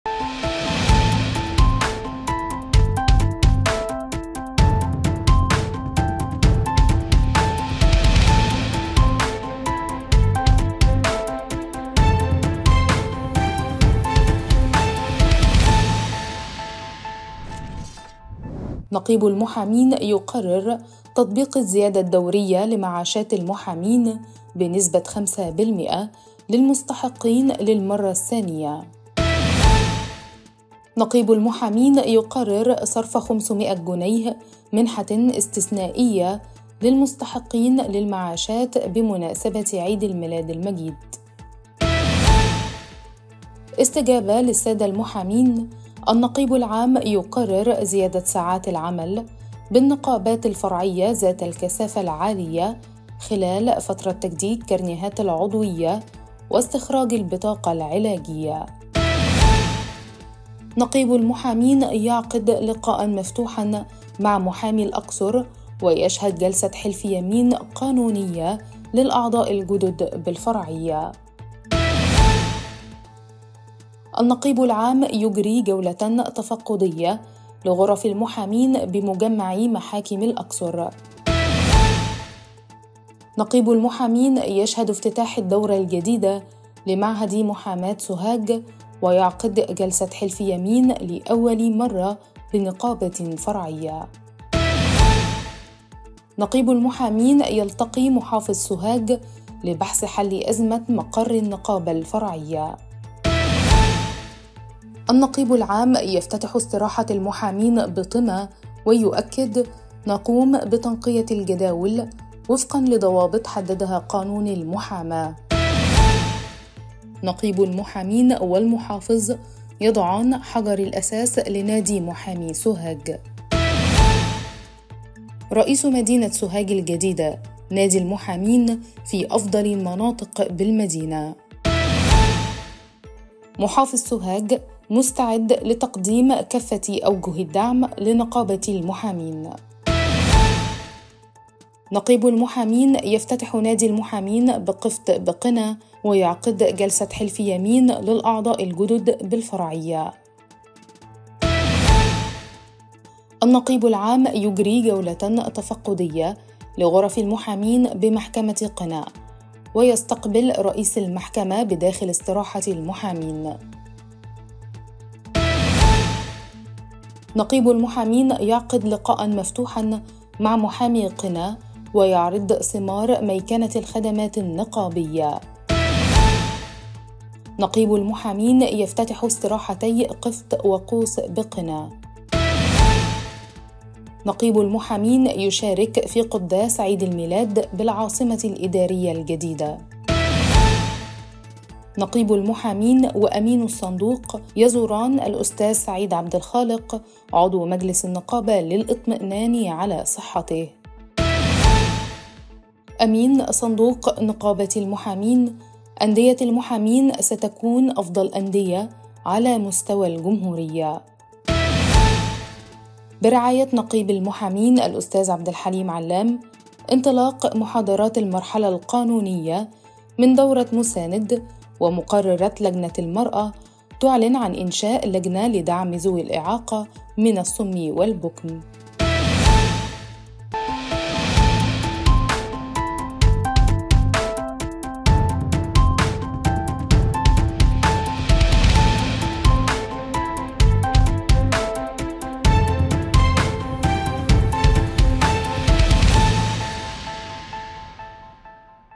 نشرة أخبار المحامين الأسبوعية من 7 وحتى 13 يناير الجاري
شهدت نقابة المحامين أحداثًا مهمة خلال الفترة من 7 وحتى 13 يناير الجاري، نعرضها في نشرة أخبار المحامين الأسبوعية، الصادرة عن المركز الإعلامي لنقابة المحامين.